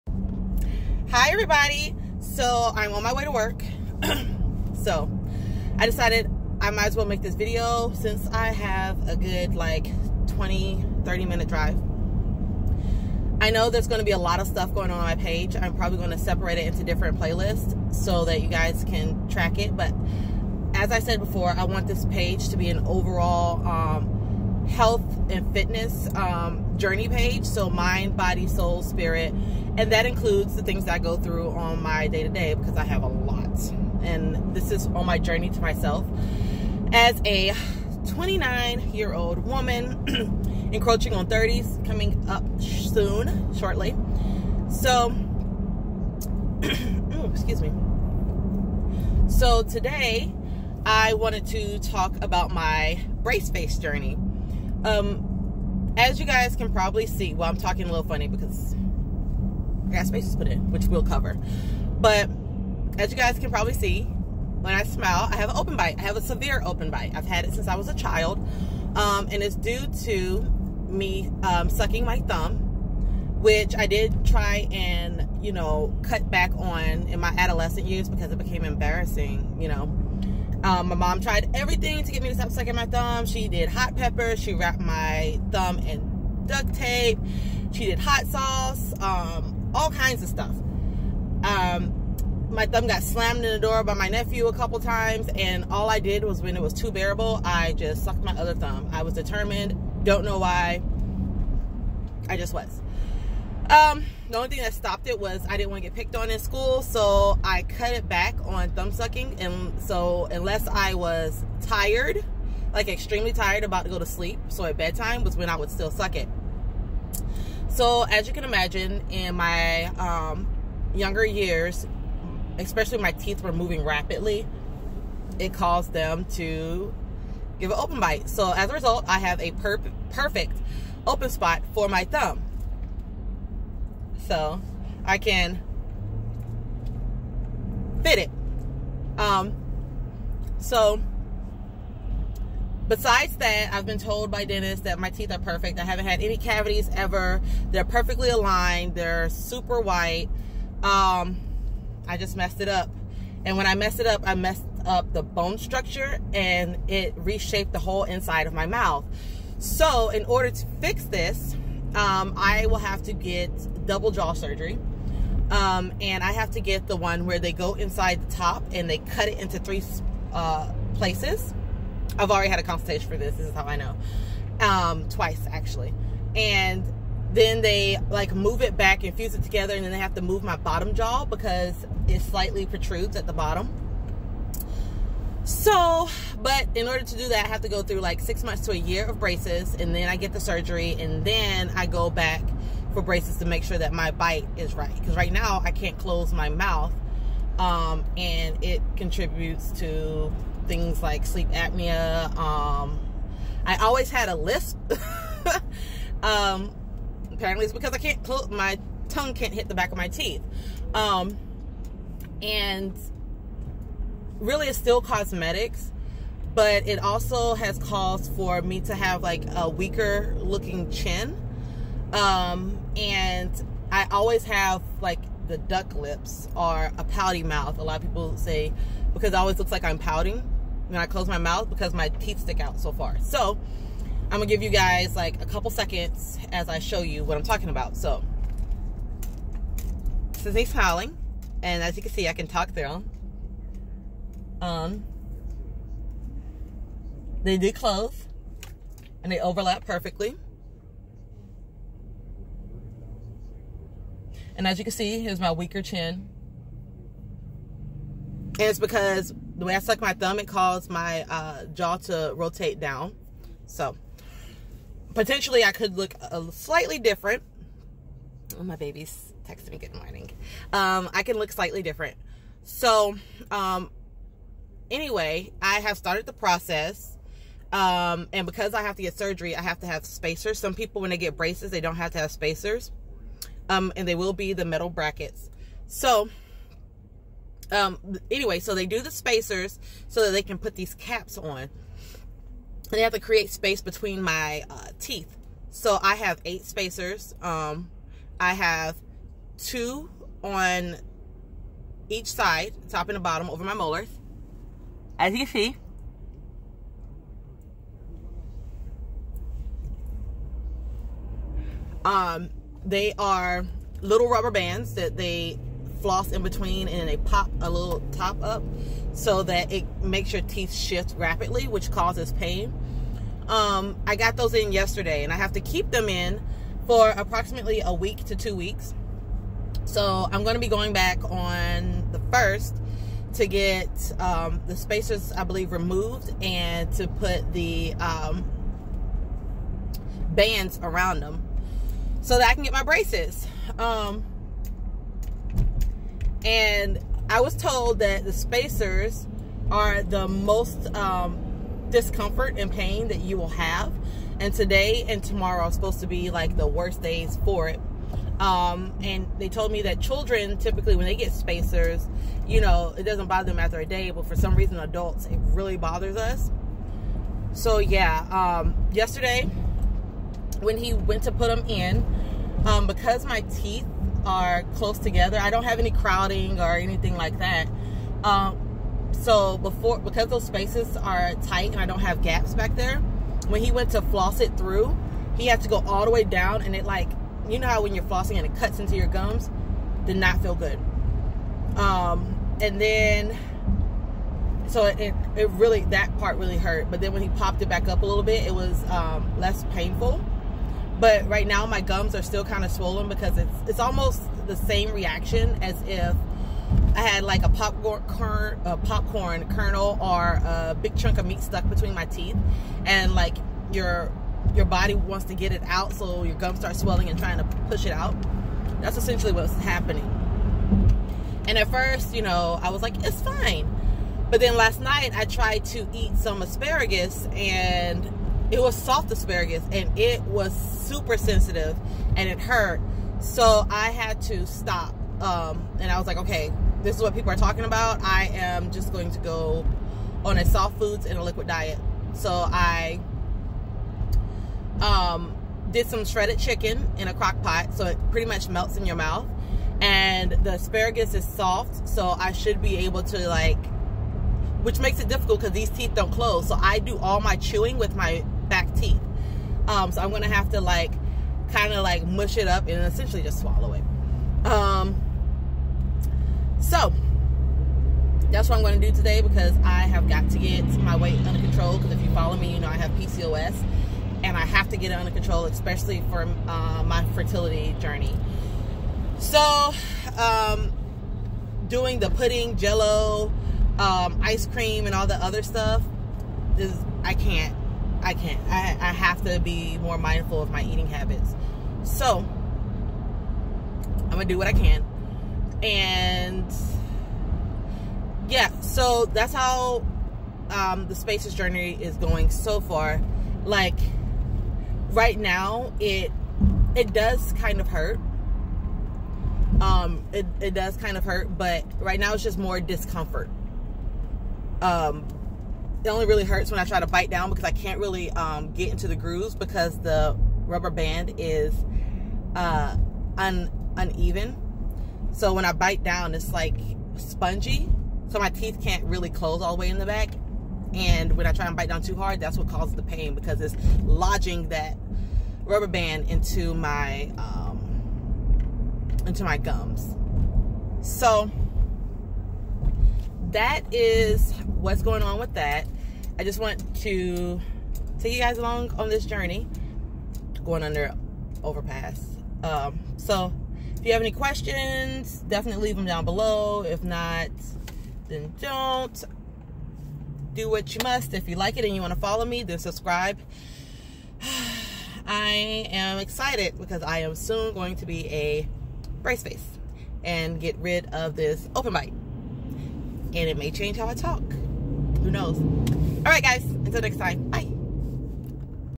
ADULT BRACEFACE || Spacers Hurt ! | Insurance paid
I got my spacers put in and THEY HURT! This whole process is free for me, due to insurance paying for it.